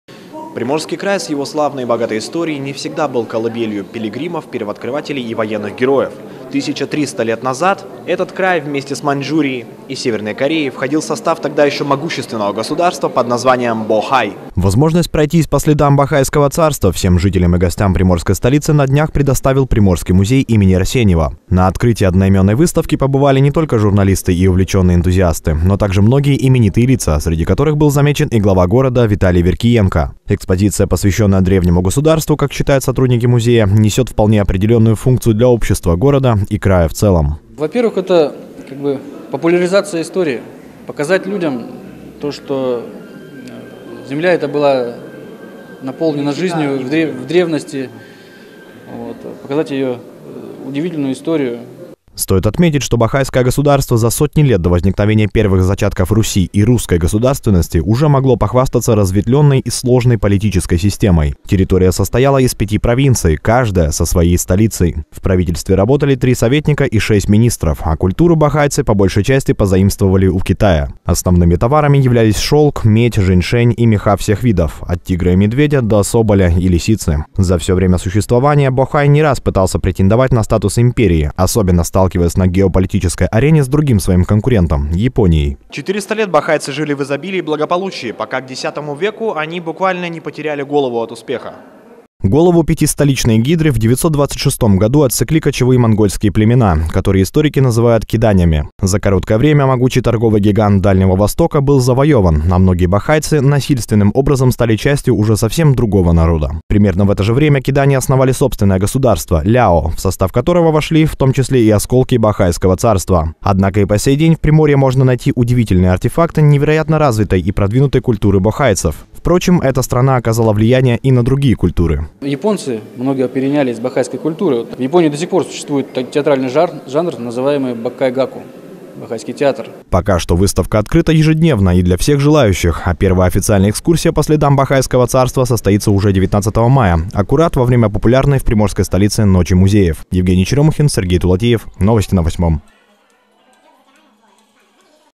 5 столиц бохайского царства
Совместная выставка Приморского музея имени Арсеньева и Института истории, археологии и этнографии народов Дальнего Востока  ДВО РАН открылась в Приморской столице. Она посвящена одной из самых загадочных страниц в истории нашего края – государству Бохай.